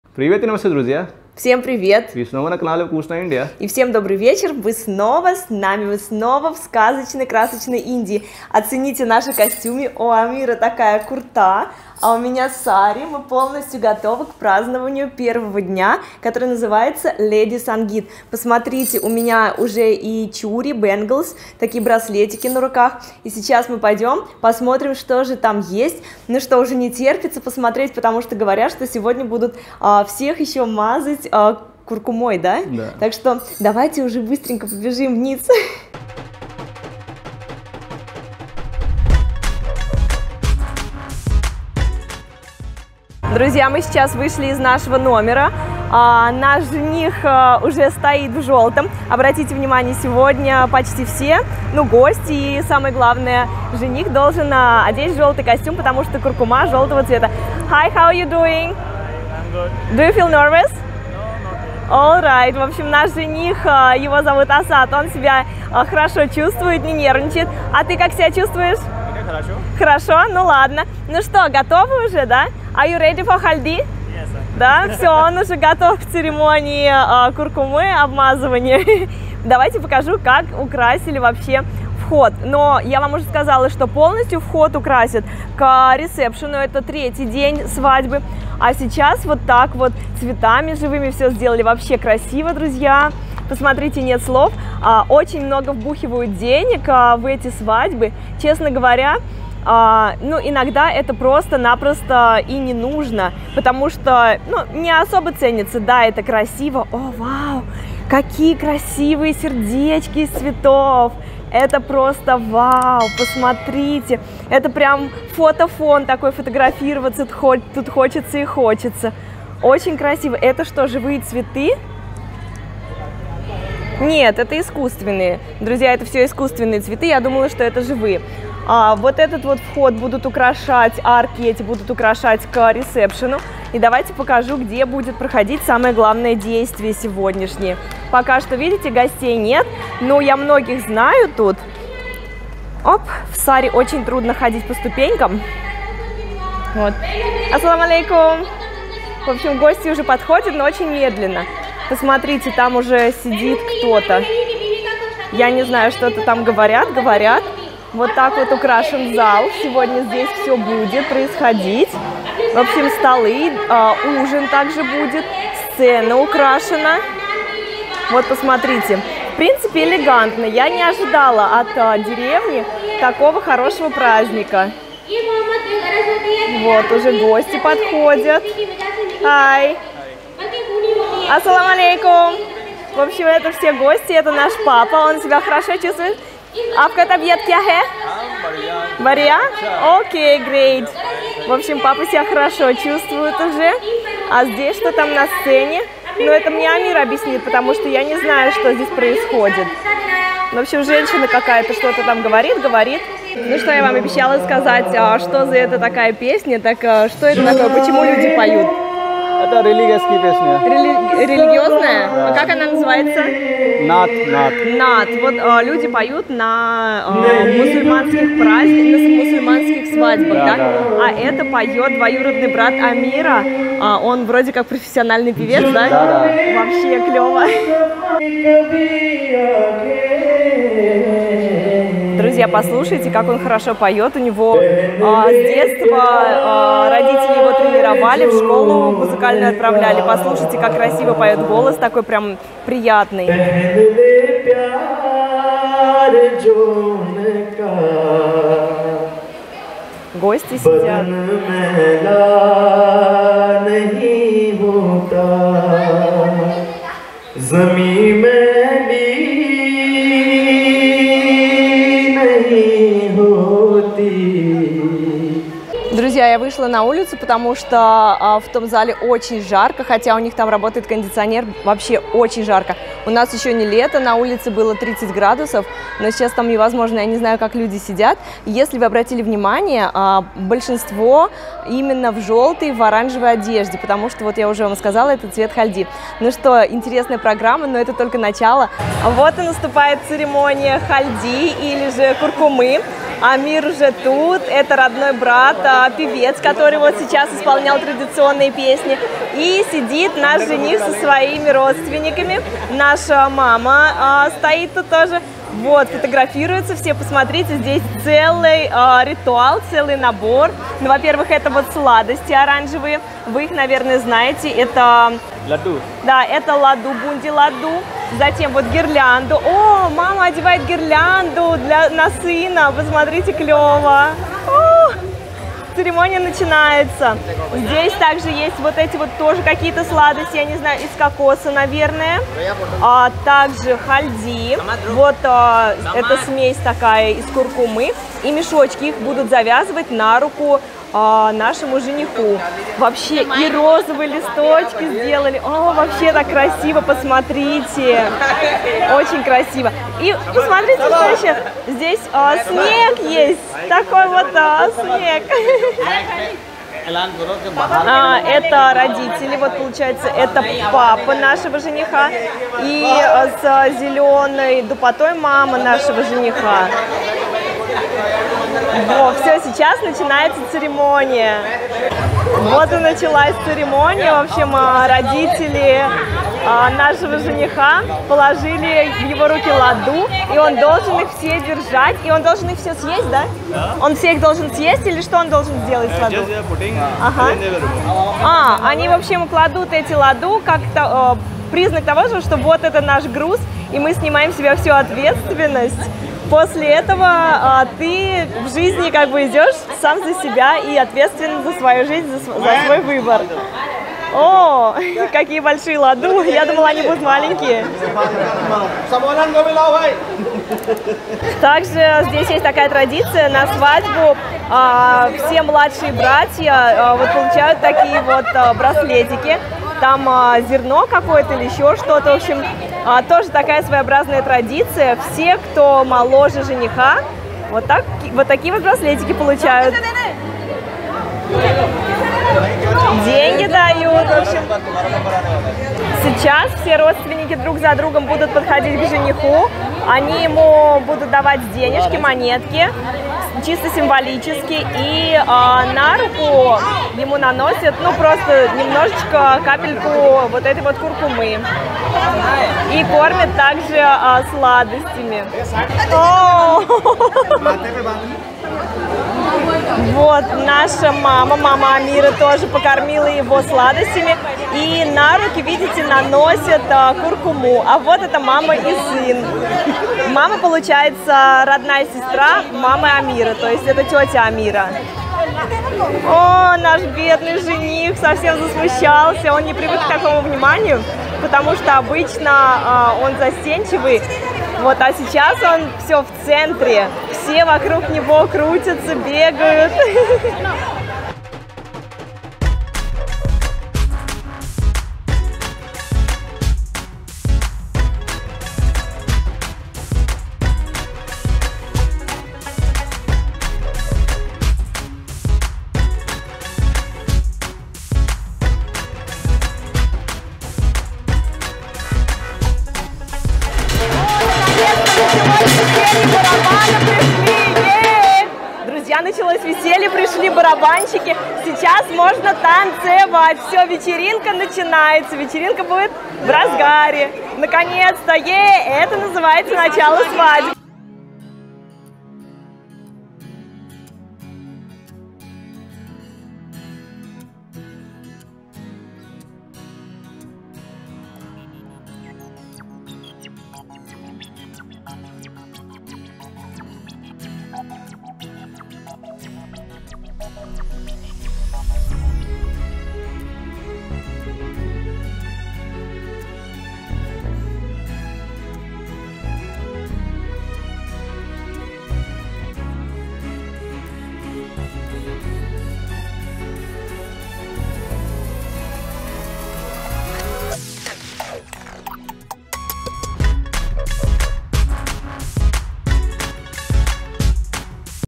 ЦЕРЕМОНИЯ ХАЛДИ- РИТУАЛЫ ПЕРЕД ИНДИЙСКОЙ СВАДЬБОЙ | СВАДЕБНЫЕ ТРАДИЦИИ | ВКУСНАЯ ИНДИЯ
ЦЕРЕМОНИЯ ХАЛДИ- РИТУАЛЫ ПЕРЕД ИНДИЙСКОЙ СВАДЬБОЙ | ТАНЦУЕТ ВСЯ СЕМЬЯ | ВКУСНАЯ ИНДИЯ
В Индии много различных свадебных традиций и ритуалов. В этом видео мы покажем вам Церемонию Халди- или церемонию обмазыкания жениха куркумой!